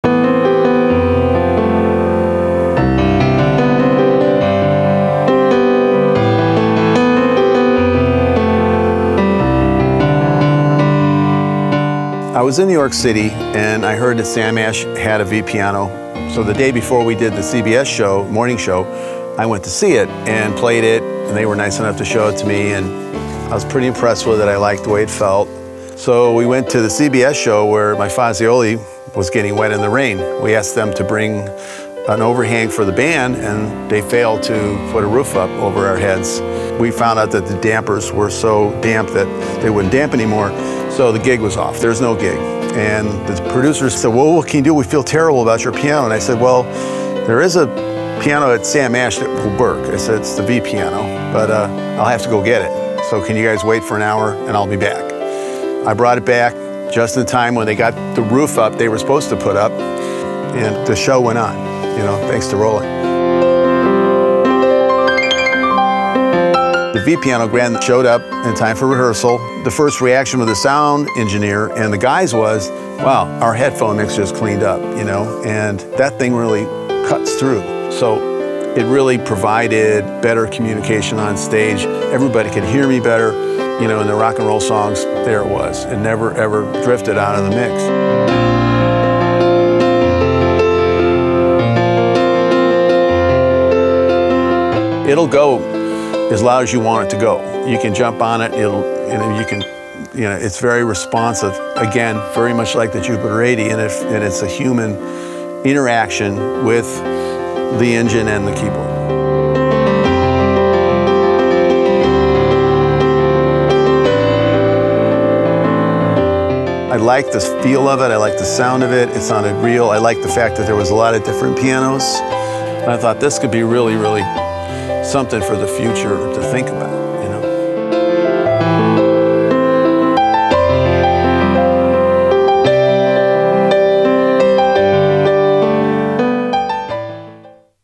Jonathan Cain on the Roland V-Piano Grand
Jonathan Cain talks about his first experience with the Roland V-Piano and how it saved his gig.